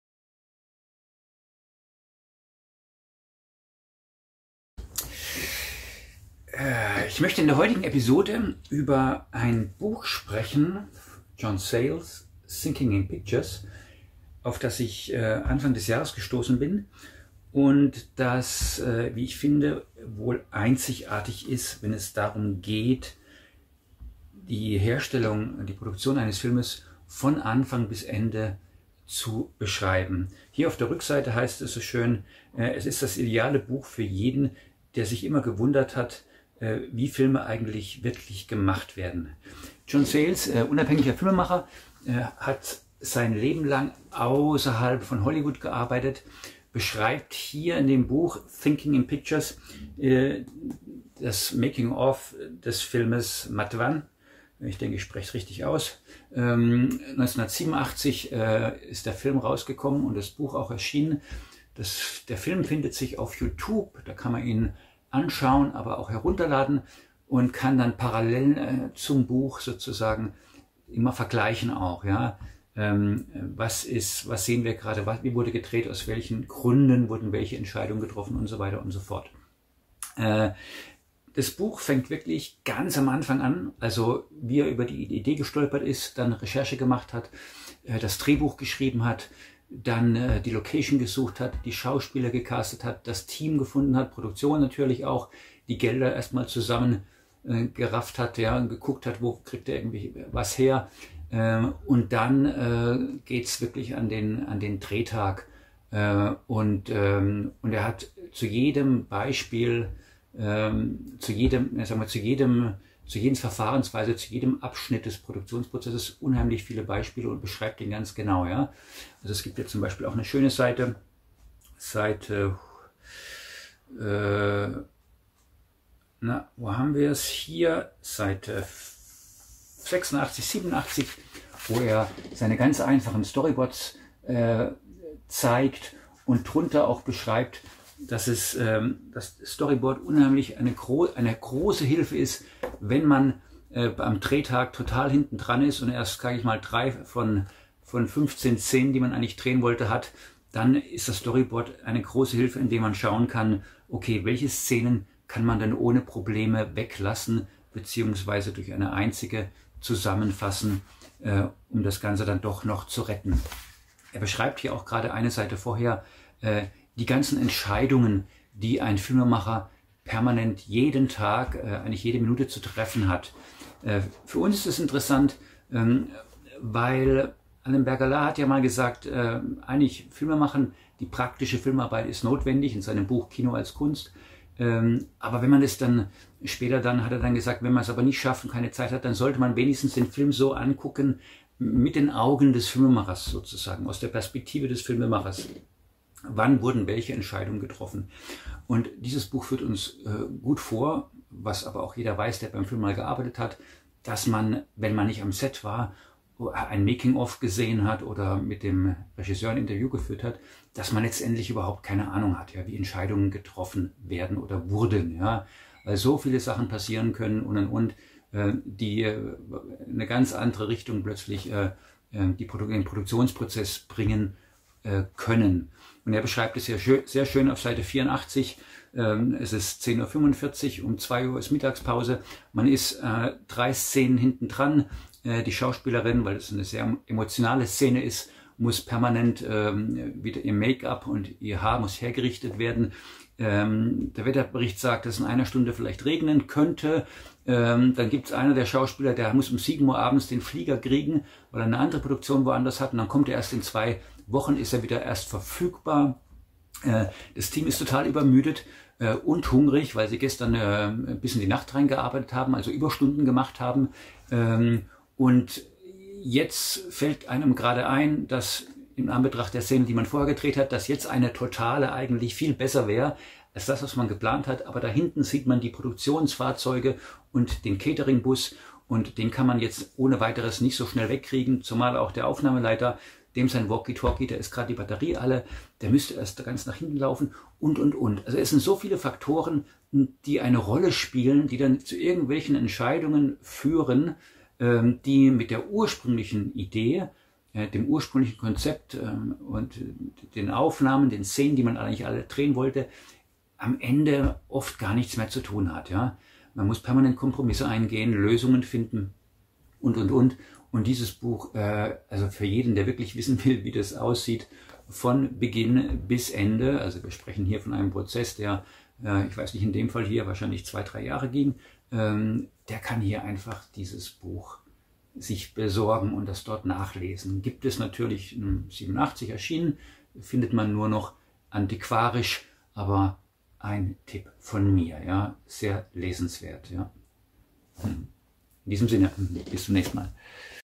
Episode 4 - John Sayles: Thinking in Pictures
John Sayles: Thinking in Pictures. The Making Of The Movie Matewan (1987)
Das Buch ist in vier Hauptabschnitte gegliedert: Denken, Schreiben, Drehen und Schneiden. Es enthält auch einige Gedanken zum fertigen Produkt sowie das gesamte Drehbuch des Films. Aus dieser Struktur ergibt sich ein Gefühl für die einzelnen Schritte bei der Herstellung eines Films und für die Art und Weise, wie alle Aspekte des Schreibens, der Produktion und der Postproduktion miteinander verknüpft sind. Der Schwerpunkt liegt dabei auf den Überlegungen und Entscheidungen, die beim Schreiben eines Drehbuchs, beim Casting eines Films, bei der Regieführung von Schauspielern, beim Aufbau eines Kurzfilms usw. angestellt werden, und zwar bis hin zur fertigen Fassung des Films.